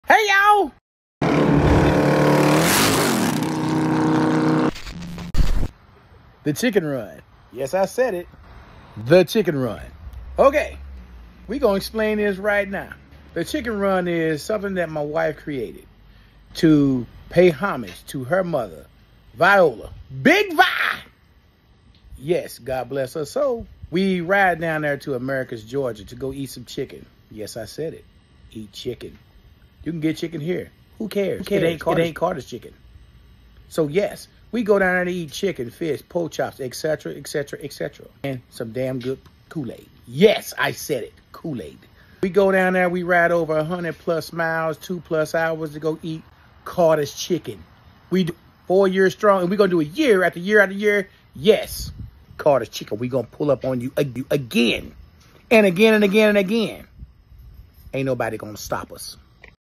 Big Viola’s Chicken Run
Big Baby and the crew head down to Americus Georgia for their annual Chicken run to Carters Chicken in memory of his wife’s mother “BIG VIOLA”